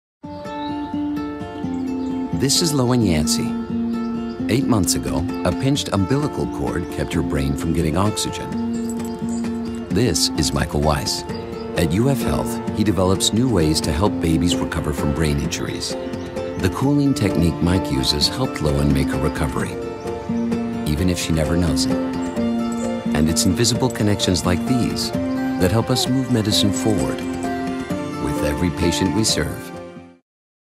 UF Health - Invisible Connections - Park
When Lowyn Yancey was born, a pinched umbilical cord kept her brain from getting
oxygen for several minutes. A cooling technique developed by Dr. Michael Weiss at UF
Health helped her brain recover, and today she's a healthy little girl. Michael and Lowyn
share an important, if invisible, connection — one that helps us move medicine forward.